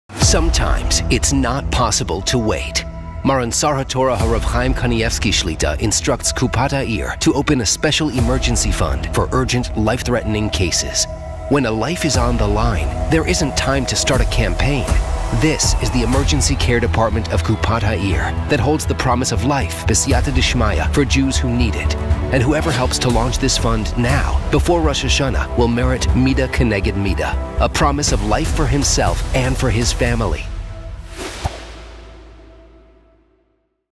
Insured for Life
The "Insured for Life" Fund: a fund that rescues lives.

The "Insured for Life" Fund is a fund that rescues the lives of people standing on the edge of the abyss. A war is raging for their lives between the Heavenly and the earthly forces, and they are suspended in between... These people are classified as "critical care," and need instant, emergency attention before it's too late. In such situations, there isn't time to even gather the family together. Saving lives is right now, this moment- or never, for that person or child whose fate hangs in the balance.

The "Insured for Life" Fund enters into the life of a family about to lose a loved one, ch"v,  and turns over the  world in order to save his life. And the "Insured for Life" Fund has merited to save lives time after time, in truly miraculous ways. When the financial aspect is no longer an obstacle, it's revealed that there is a lot more that can be done for people who originally seem like they are about to leave this world; and sometimes they stay, and even recover.

 You are insuring the lives of other and Maran Sar HaTorah HGR' Chaim Kanievsky signs on the "Insured for Life" certificate- in order that you merit a good, long, and peaceful life.

This fund's premise is 'midda k'neged midda.' You merit to save a life- that without you, apparently wouldn't be here, and Hashem will save your lives from such a din. A life for a life- you save Hashem's child, and Hashem will save your child. It's as simple as that.

Maran Sar HaTorah HGR' Chaim Kanievsky shlit"a initiated this certificate a few years ago. At midday on erev Rosh Hashana, an entire entourage entered his home for the 'Hataras Klalos' ceremony, which was the 'sur mi'ra' [turn away from bad] part; and then, at the same gathering, the 'asei tov' [do good] part- they signed the Certificate of Life.

You, the contributors, sign on your section- you are insuring the lives of others. In this section, also Maran Sar HaTorah HGR' Chaim Kanievsky shlit"a does his part- he signs on the second side, signs on the "Insured for Life" certificate- in order that you merit a good, long, and peaceful life.

Whoever ensures that others will live- will merit to have this certificate, the certificate of Insured for Life. This certificate will 'stand in the breach' against the middas hadin, and also nullify unfavorable decrees.

This certificate is our chance to pass safely through the Yamim Noraim, and to come out with a truly favorable judgment- a judgment of life. A long, blessed life, full of satisfaction and happiness, with authentic Jewish nachas, with health and abundance.